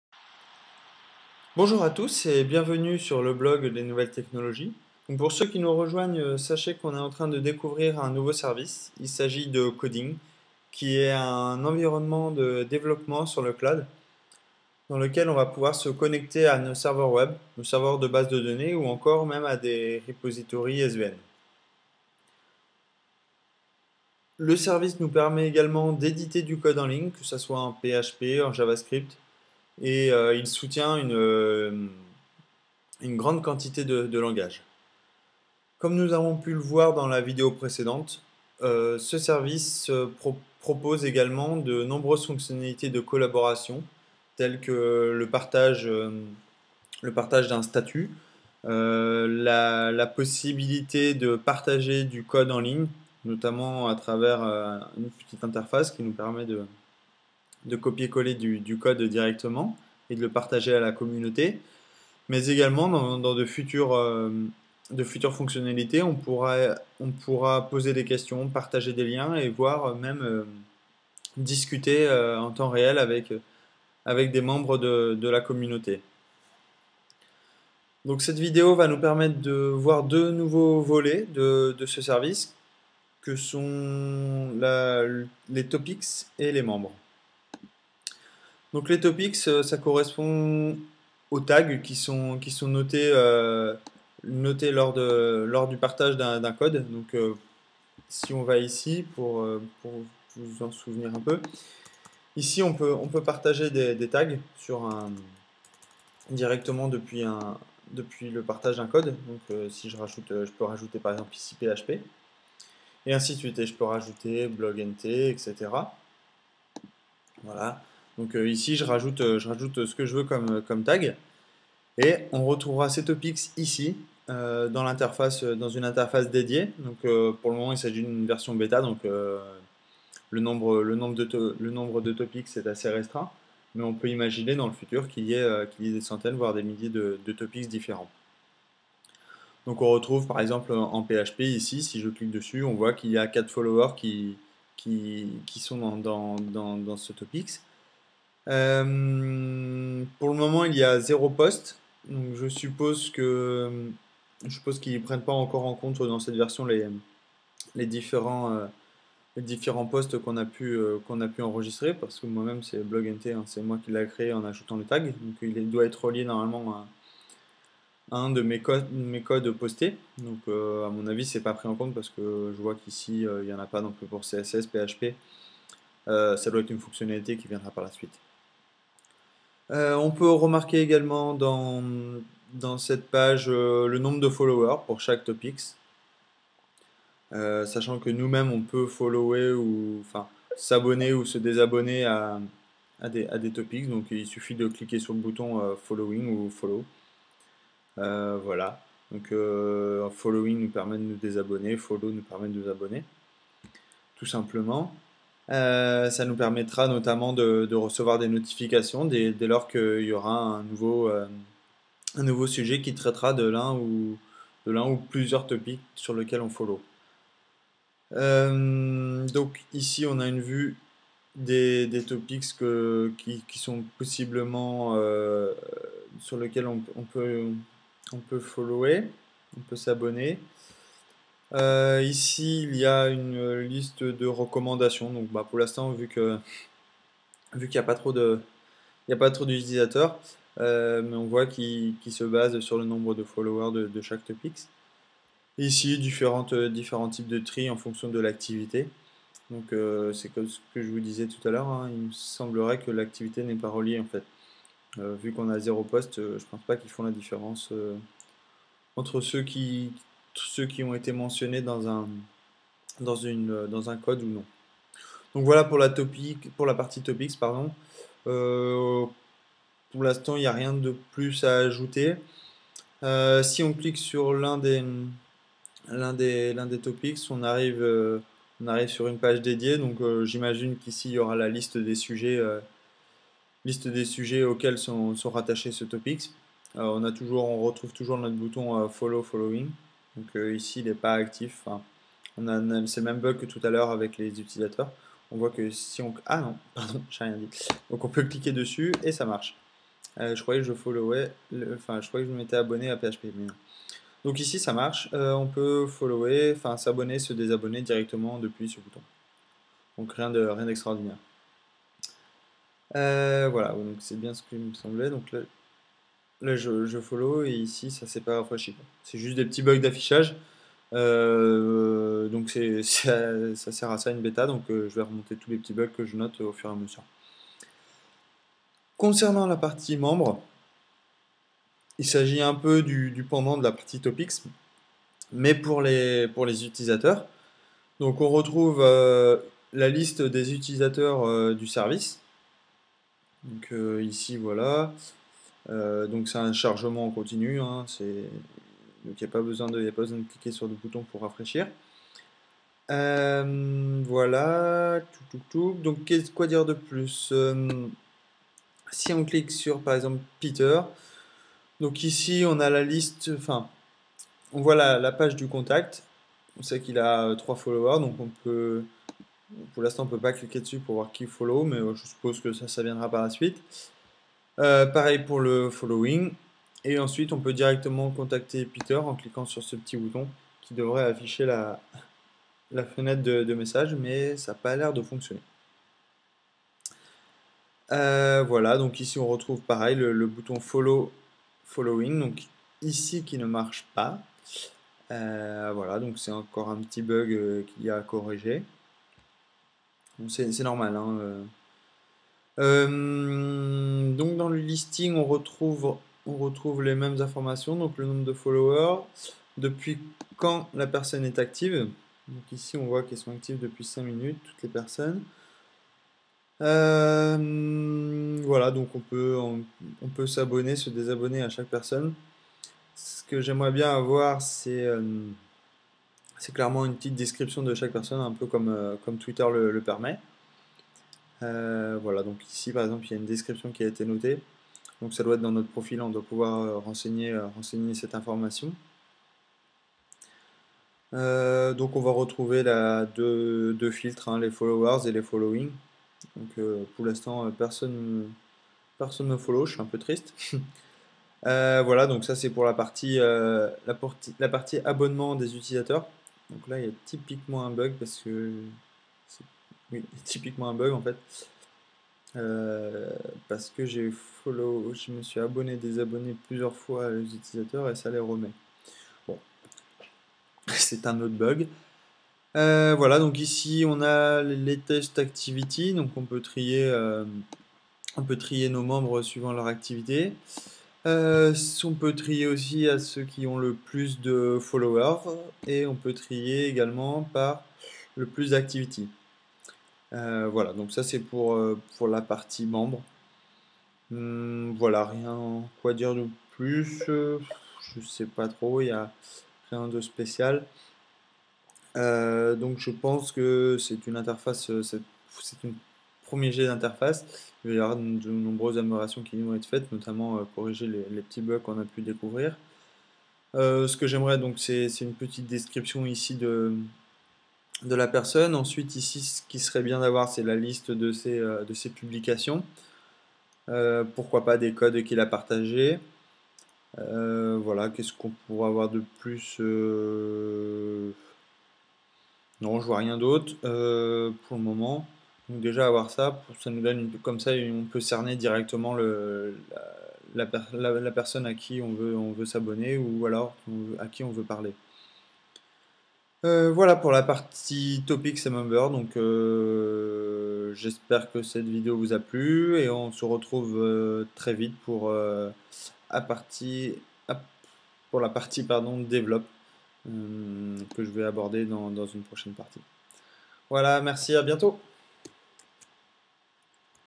Koding : Découvrez la fonctionnalité « Topics / Members »
Koding est un environnement de développement sur le cloud. Dans cette vidéo vous allez découvrir la partie "Topics/Members" du service.